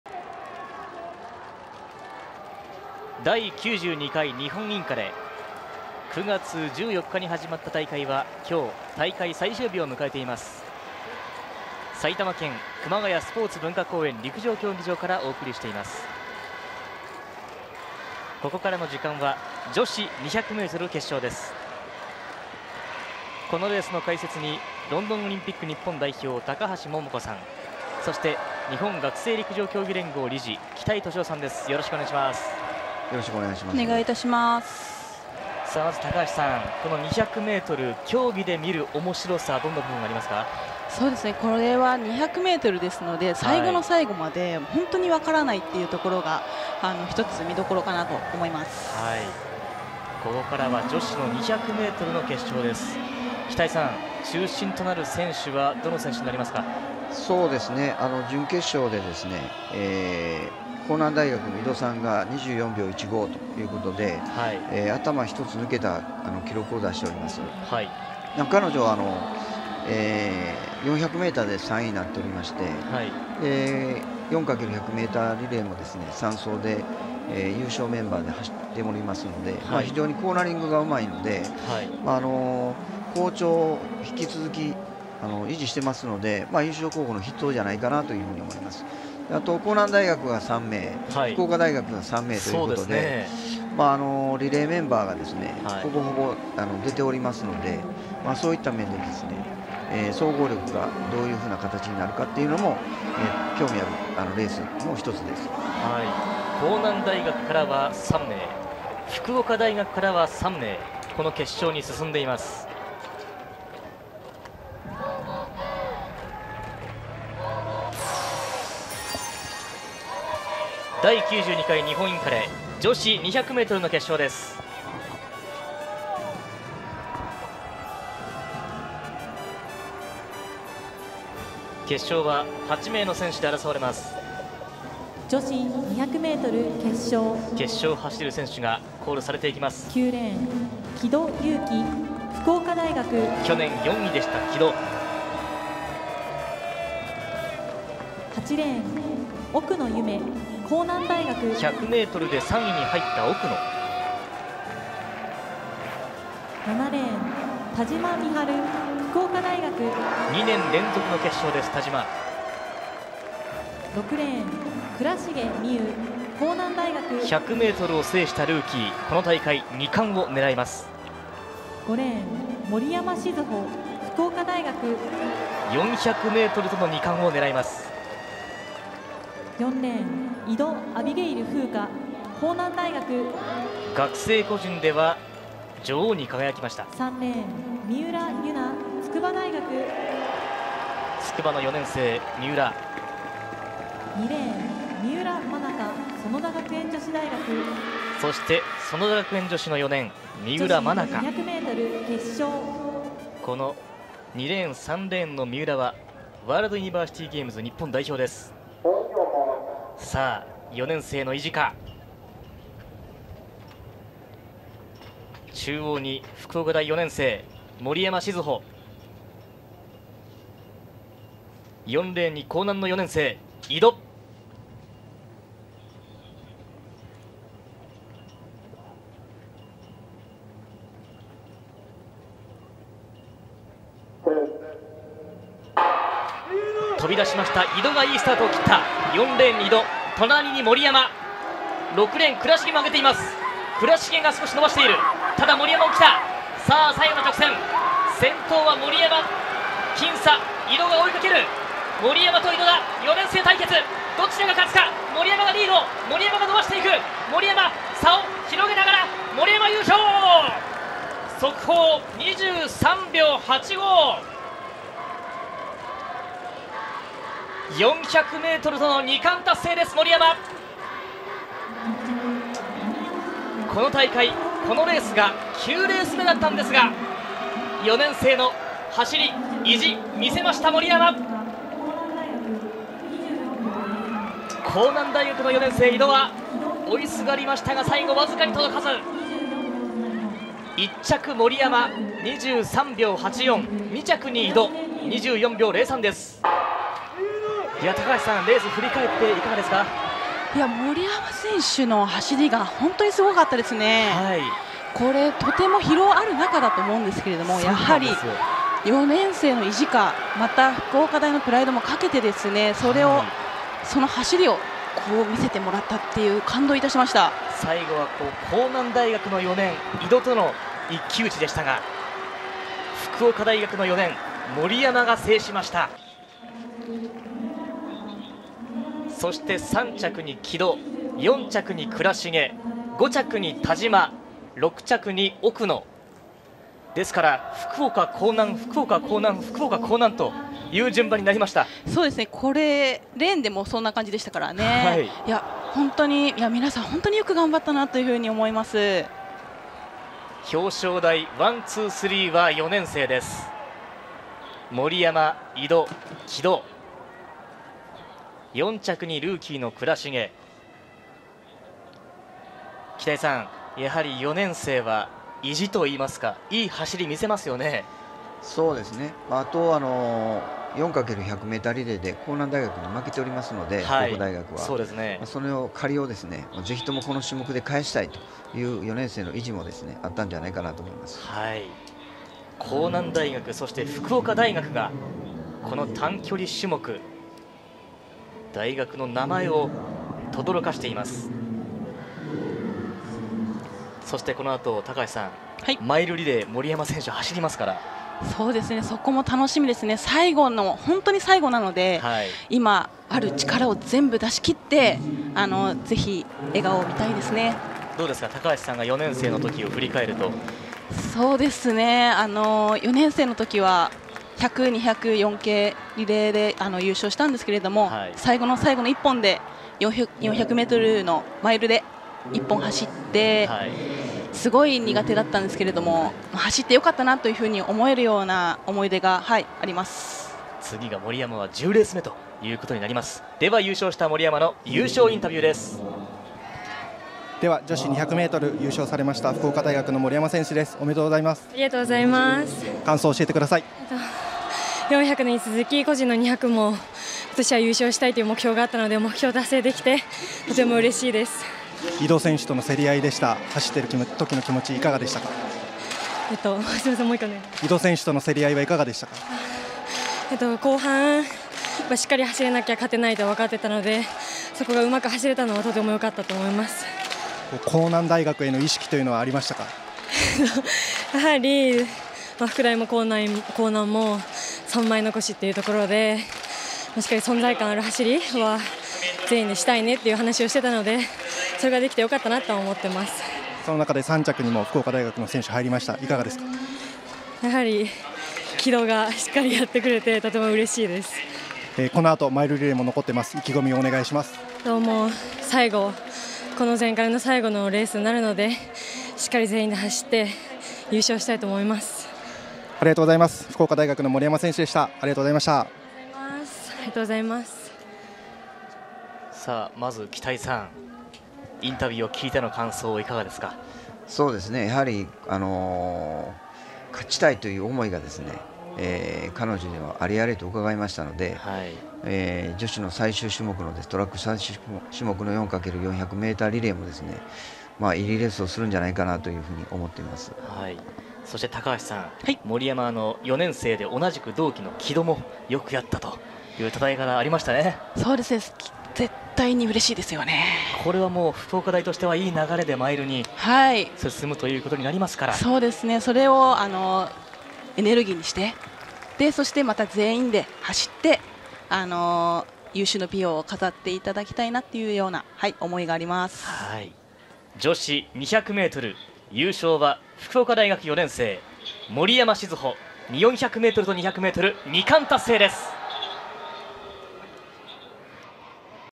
【第92回日本インカレ】森山静穂 2冠達成！（200m/400m）｜女子200m 決勝
【配信協賛】
センコー株式会社

【配信協力】
日本学生陸上競技連合

(C)ytv